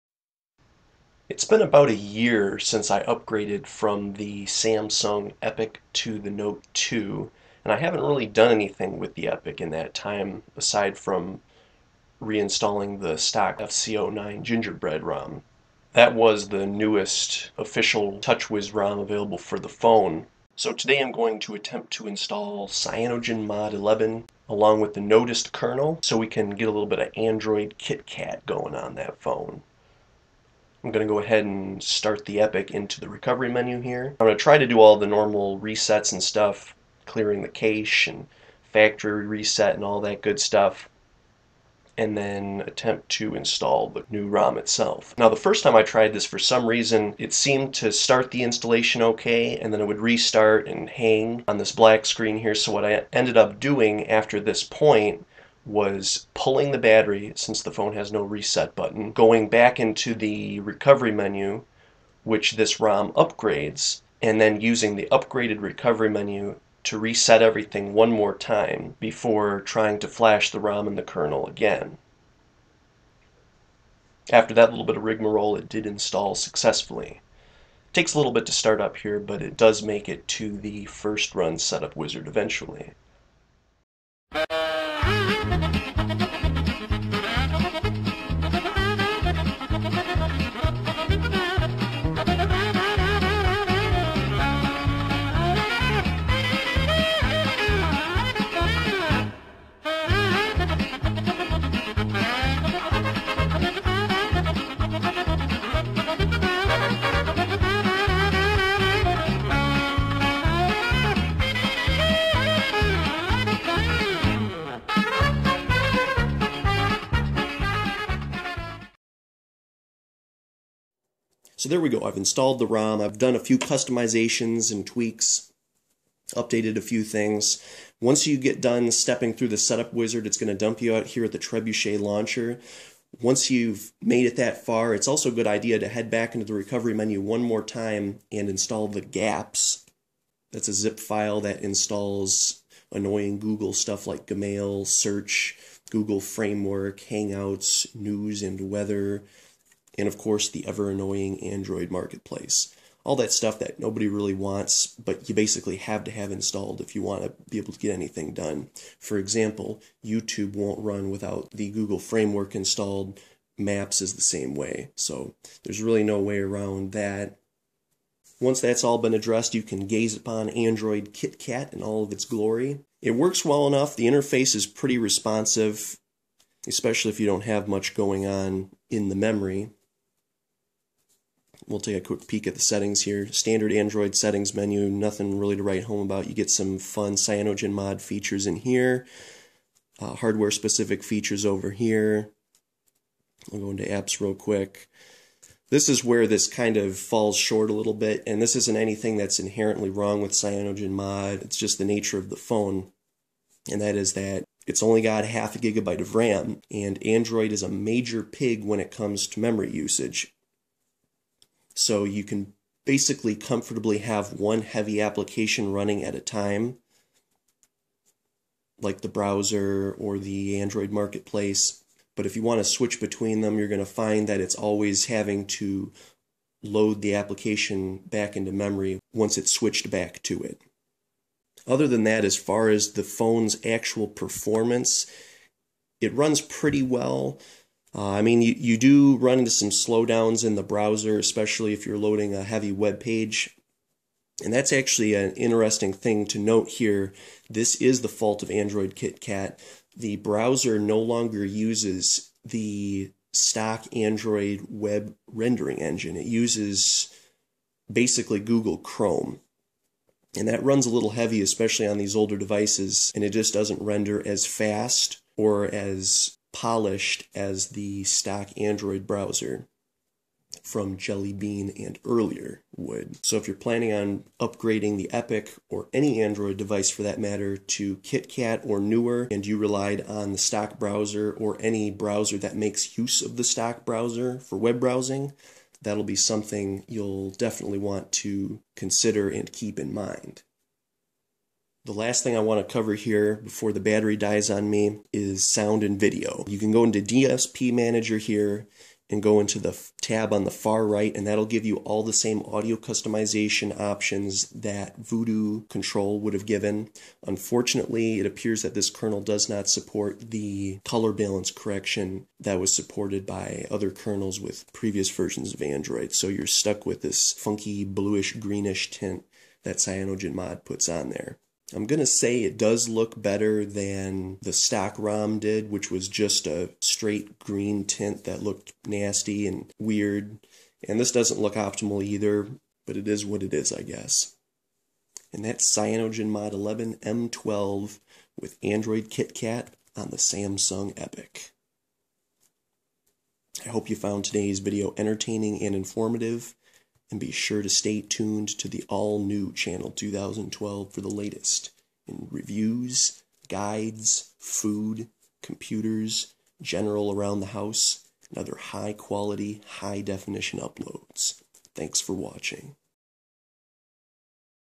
Samsung Epic CM11 KitKat Installation and Demonstration
To celebrate my one year anniversary of retiring the Epic, I will be installing CyanogenMod 11 M12 and the Notest Kernel on the device. Today's video will cover installation tips and a brief overview of everything you need to know about KitKat on the Samsung Epic.

Alternatively, if you'd like to use something a little lighter and easier on this phone's 512MB of RAM, CM9 (Ice Cream Sandwich) runs pretty well on the Epic as well. Here's the links for CM9.

These links were last updated in July of 2017. If any of them go bad for whatever reason, do not hesitate to comment on this video or send me a personal message so that the issue can be corrected.